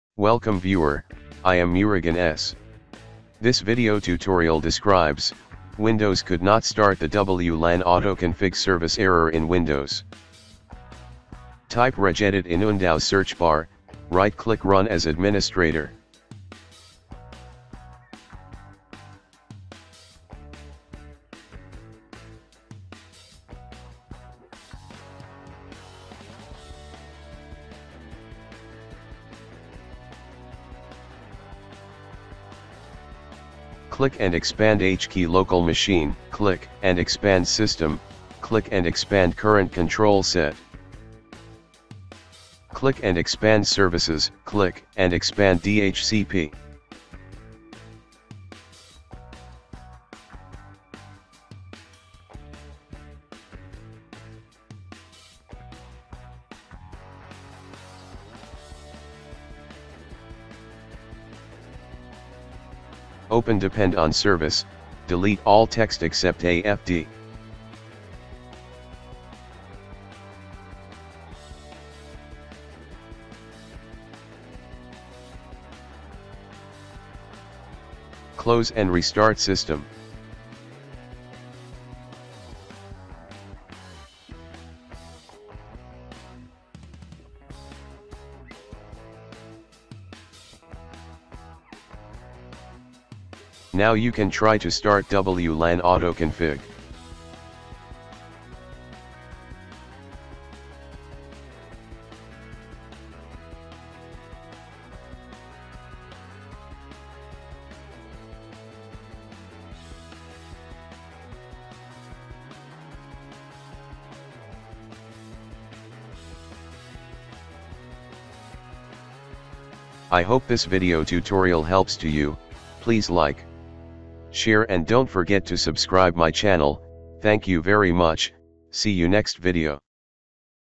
Windows could not start the WLAN AutoConfig Service Error In Windows 11/10/8/7
This Tutorial helps to Windows could not start the WLAN AutoConfig Service Error In Windows 11/10/8/7

00:00 Intro
00:11 Open Registry Editor
01:04 Open Depend on Service
01:25 Close & Restart System
01:47 Start WLAN AutoConfig
01:59 Closing